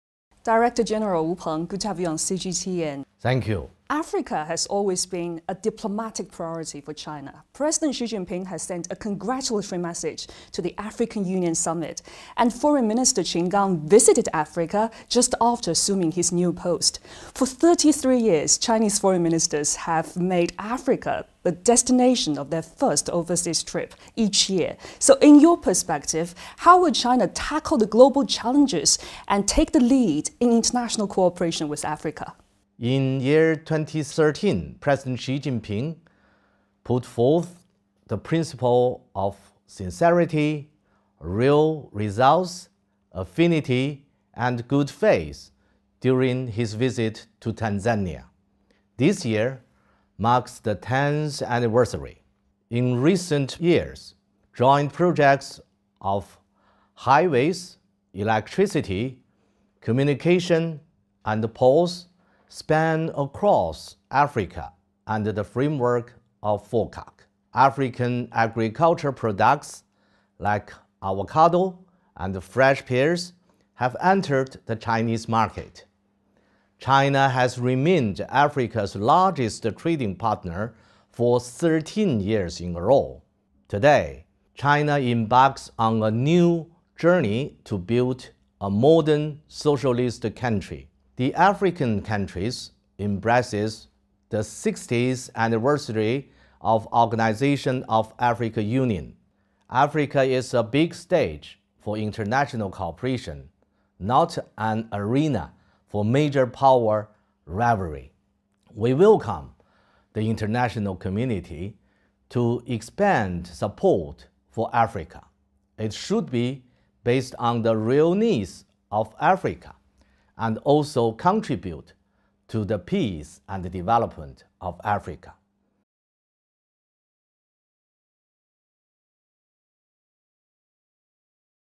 Wu Peng: China-Africa cooperation is open and inclusive
In the "High-Talk" segment, we speak with some of the nation's brightest minds on key global issues. Today, CGTN anchor Su Yuting speaks with Wu Peng, Director-General of the Chinese Foreign Ministry's Department of African Affairs, on China-Africa cooperation.